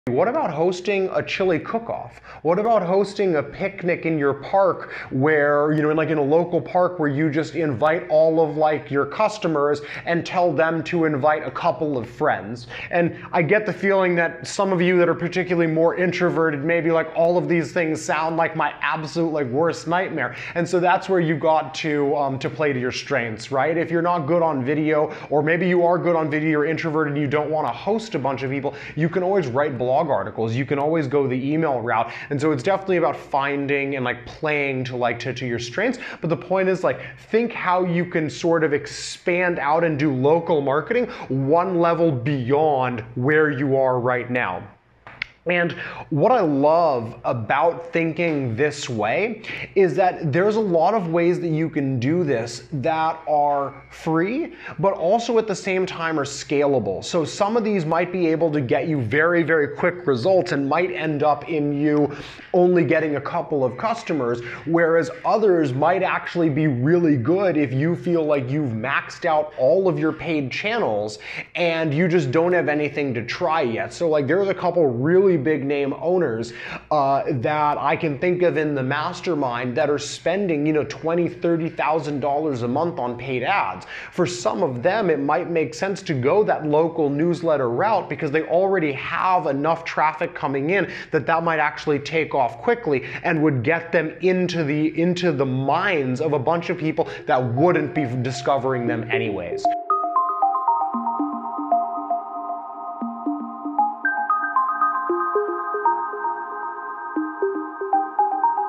Maid Service Marketing Tip #2: Throw A Party!
Ever thought of throwing a party to promote your Maid Service?

Cleaning industry and marketing expert, Amar Ghose, talks about throwing a get-together in the community in order to build your cleaning business’ network.

Amar Ghose is the CEO and Co-Founder of ZenMaid, a Maid Service scheduling software company.

This awesome talk was just one of 44 expert talks shown in the 2019 Maid Summit - the first & online virtual conference for Maid Services.